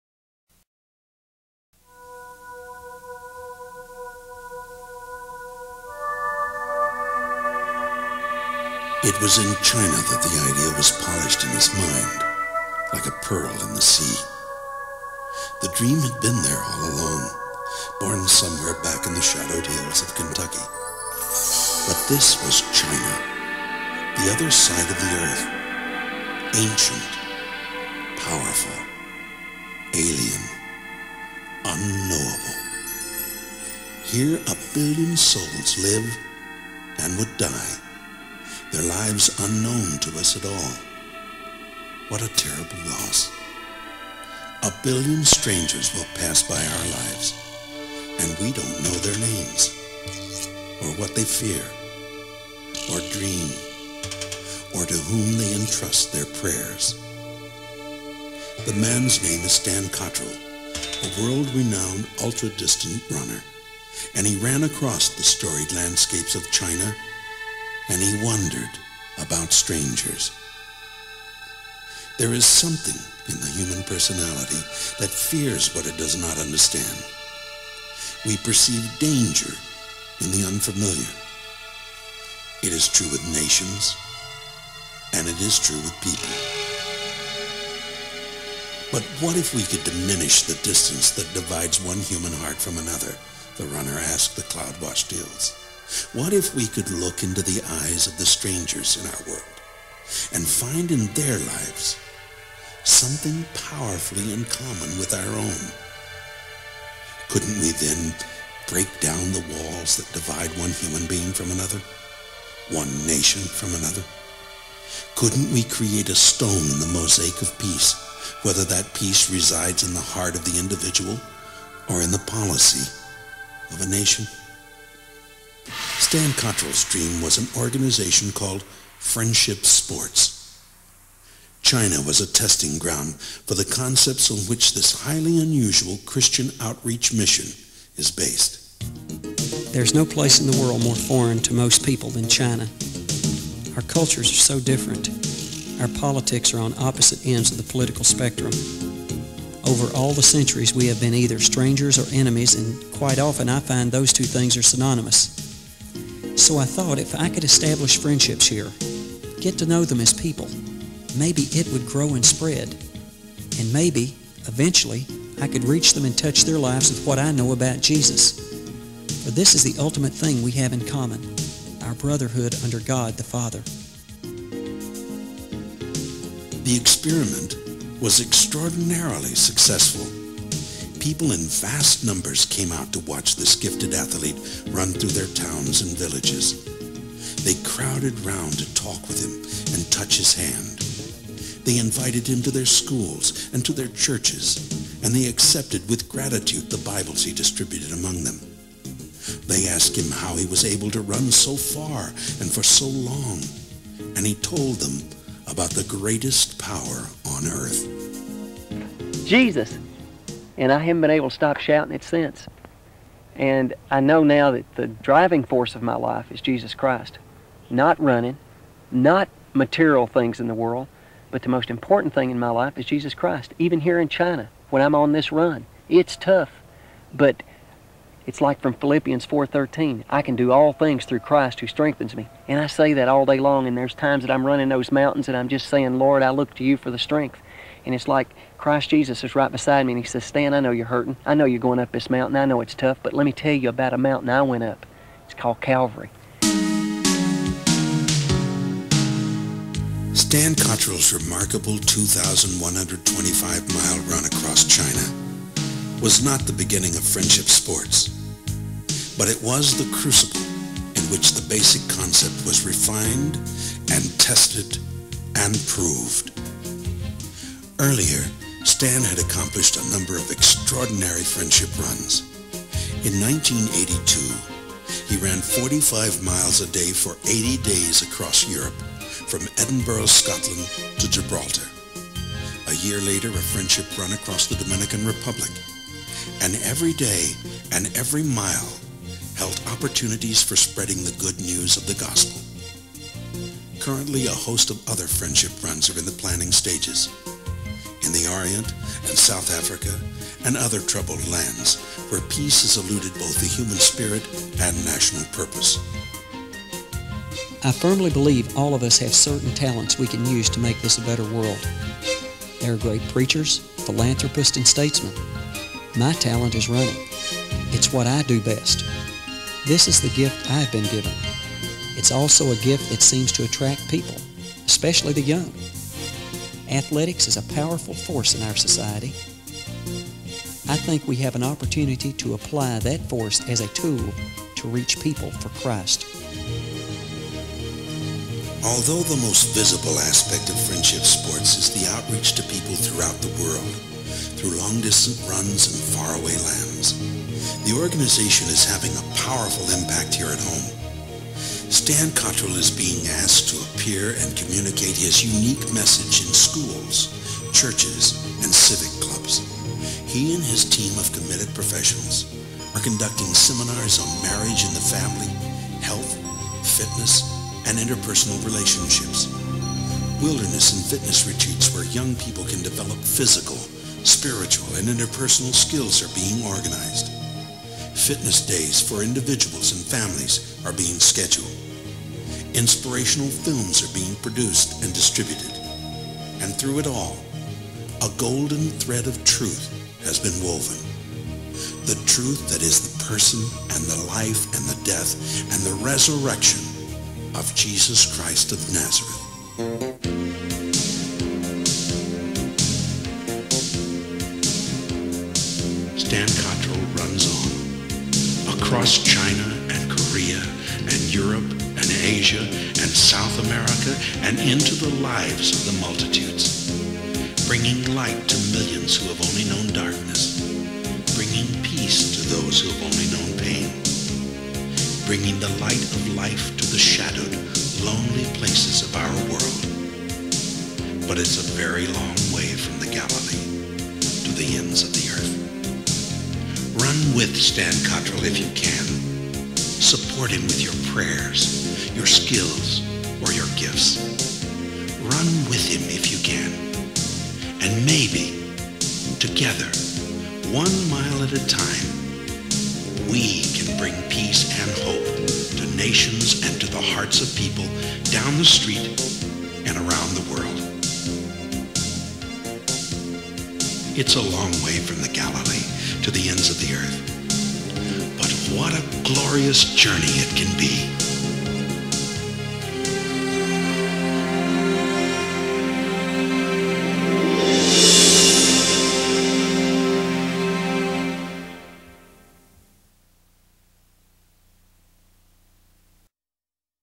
Friendship Sports - Stan Cottrell
-- World renowned ultra distance runner, Stan Cottrell's premise in founding Friendship Sports Association was tested and proven through his 2125 miles of runs through the mysterious, remote areas of China in 1984. This short Mickey Grant production expresses the driving force, the heart, mission and effectiveness of Stan Cottrell's "Friendship Runs" throughout all the world, and why The Great Global Friendship Run is a "race" that MUST be run!... "We fear the unknown," but Stan Cottrell sheds light with understanding, breaks down walls that divide one human heart from another, spreads hope, and is raising funds for the 143 million orphans in the world... GlobalRun GGFR ...

Come and join us!... Contact through to help - Keep track through - Contribute through - Spread the word through your social media networks -- And pray for us! -- Stay tuned: Mickey Grant will be filming it all!